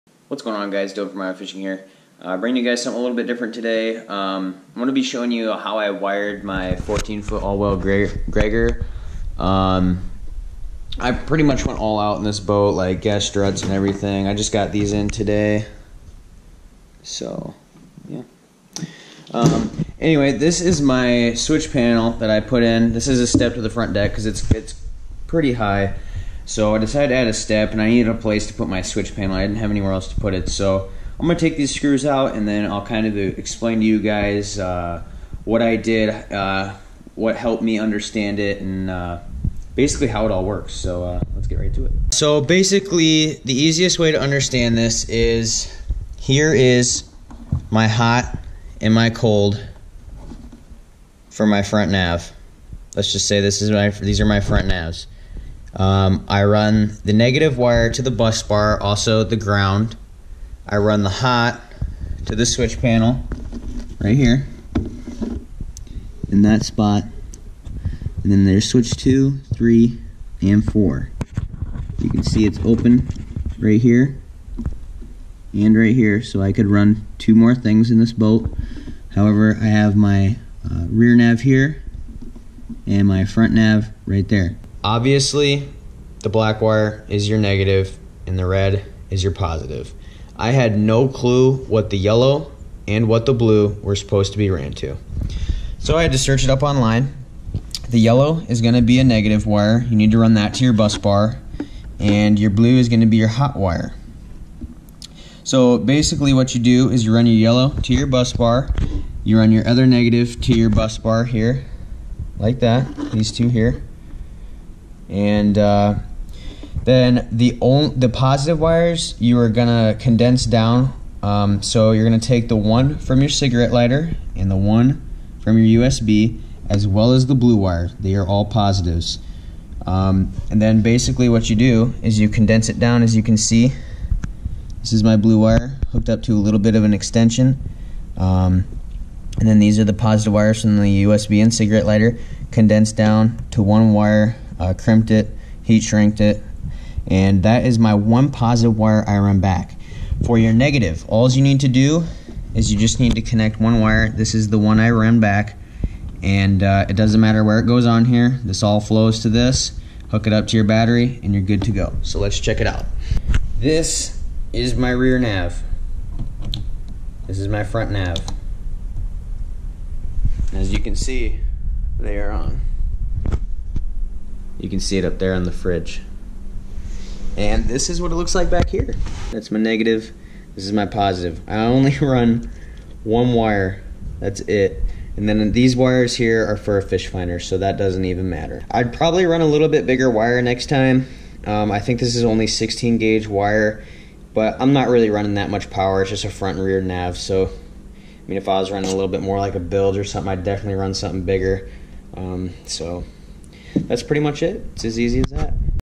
Gregor Project: Electrical Walk Through - [HD]
Here is the first video I've put out showing the new bass boat I've been working on. In this video I will be showing you how I ran my electrical and how it all works. It looks like a mess, but it's some clean wiring! Hope this helps you guys out. Full boat tour video coming soon!!